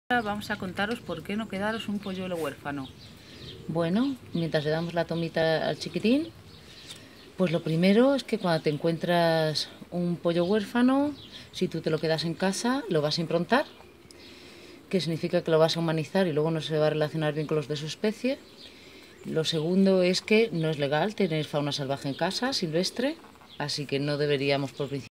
Por qué no quedarse pollos huérfanos en casa
Acércales al centro de rehab. de especies silvestres más cercano o a un veterinario especialista en aves. Su vida merece especialistas.